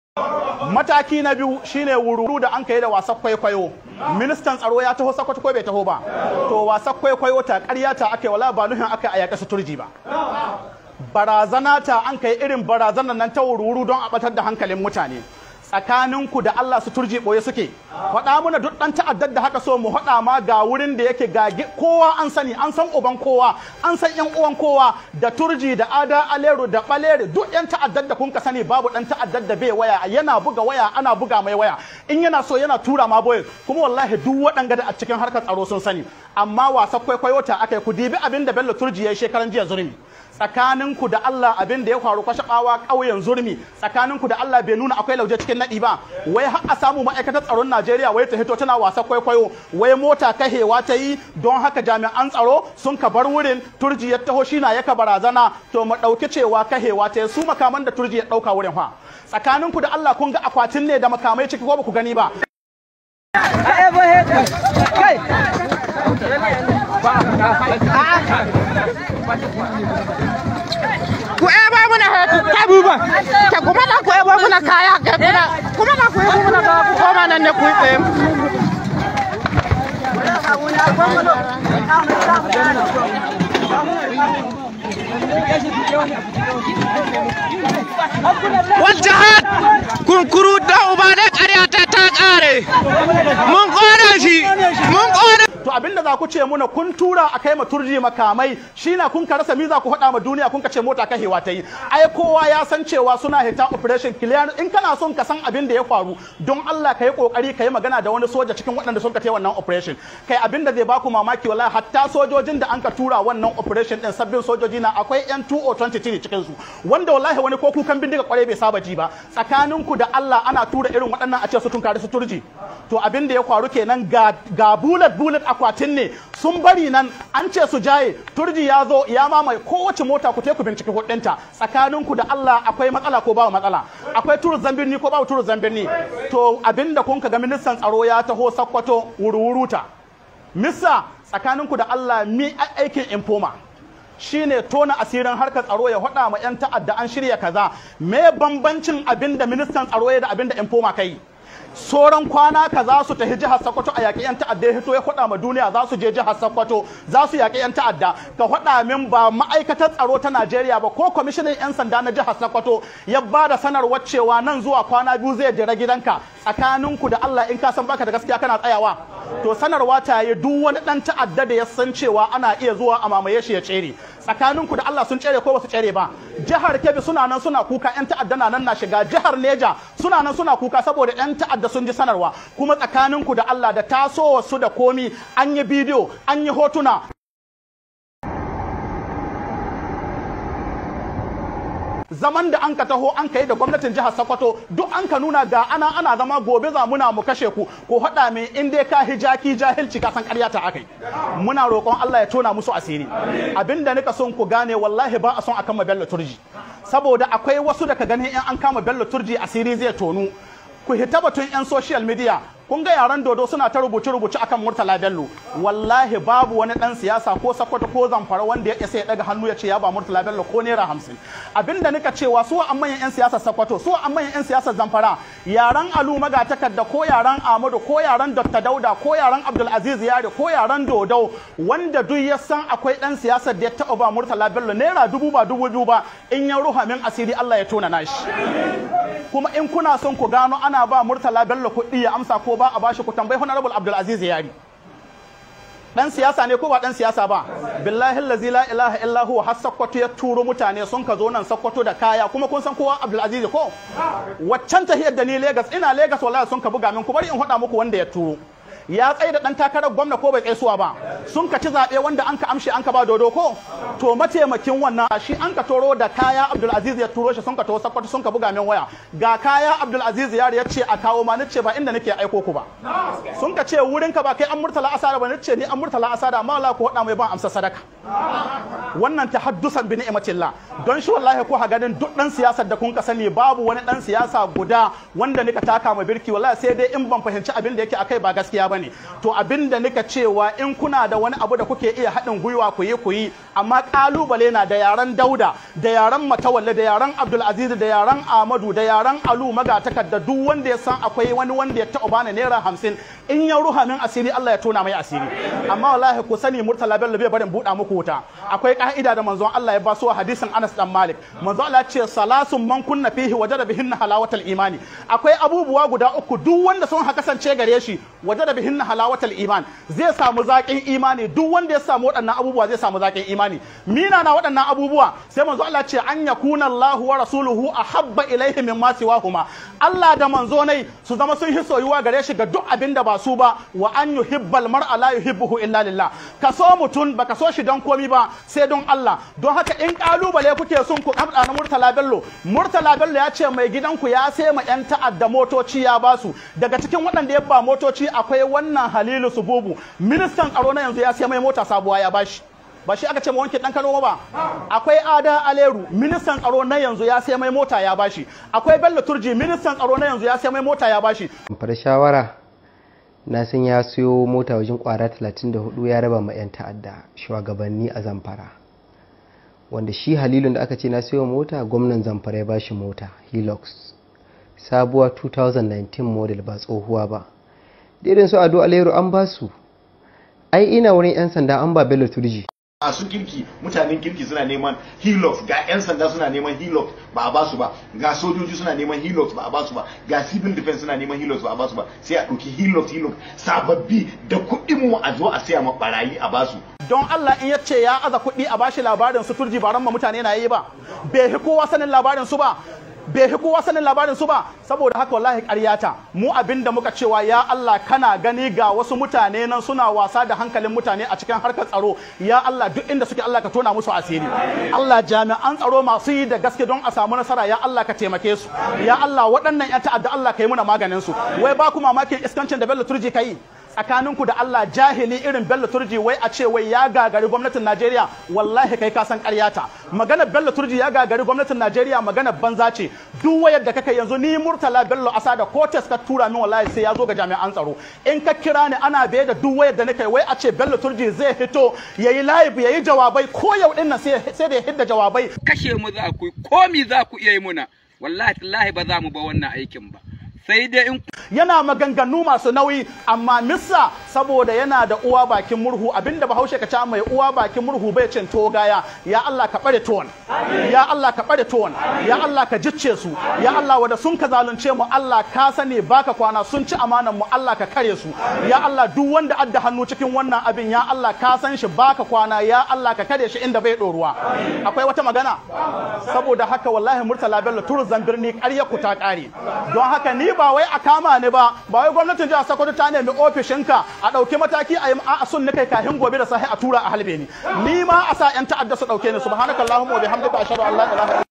Cin Amanar Tsaron Arewa | Daga Bakin Sheikh Murtala Bello Asada
CIN AMANAR TSARON AREWA

Daga bakin Sheikh Murtala Bello Asada, yadda akayi amfani da Sojojin Nigeria aka kaiwa B£||0 T_urji makaman yaki

Da kuma yadda Ministan Tsaro Bello Matawalle ya wargaza shirin da akayi na yakar 'yan ta'adda a Sokoto

Dama na fada zuwan Ministan Tsaro Sokoto, nace yaudara ne kawai da wasan kwaikwayo da zubar da kima da martaban rundinar Sojin Nigeria

Ina maimaitawa; wai shin har yaushe ne rundinar Sojin Nigeria zata dena bari miyagun 'yan siyasa suna shiga cikin kakinta suna siyasa da harkan tsaro?

Idan martaban rundinar Sojin Nigeria ya zube to me ya saura a Kasar na inganta tsaro?

Yaa Allah Ka mana maganin masu cin amanar tsaron Arewa